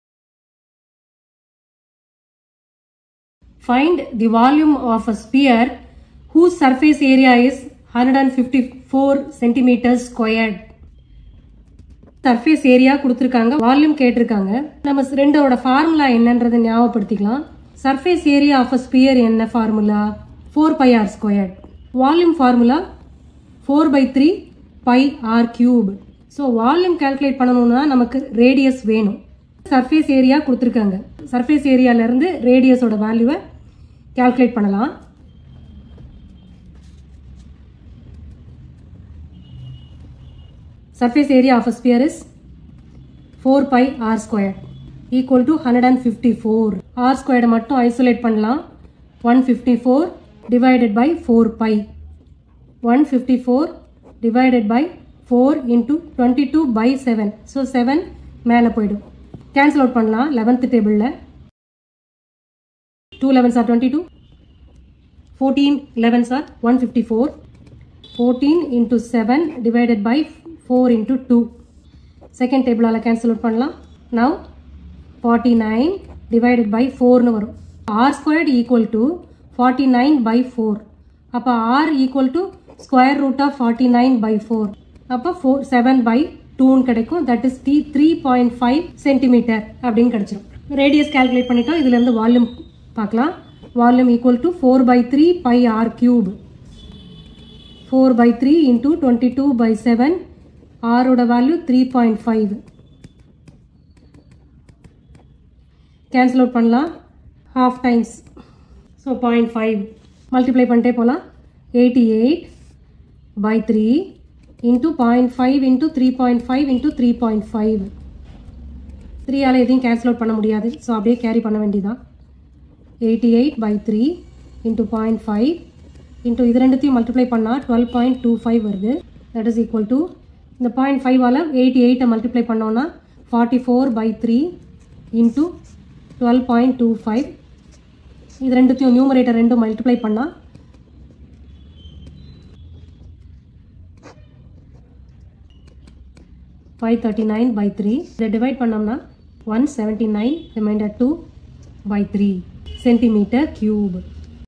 CLASS 9 CH 11 SURFACE AREAS AND VOLUMES EXERCISE 11.4 QUESTION 7 PAGE 150 NCERT (In Tamil) - Mrs.DB
Class 9 Surface Areas And Volumes Exercise 11.4 Question 7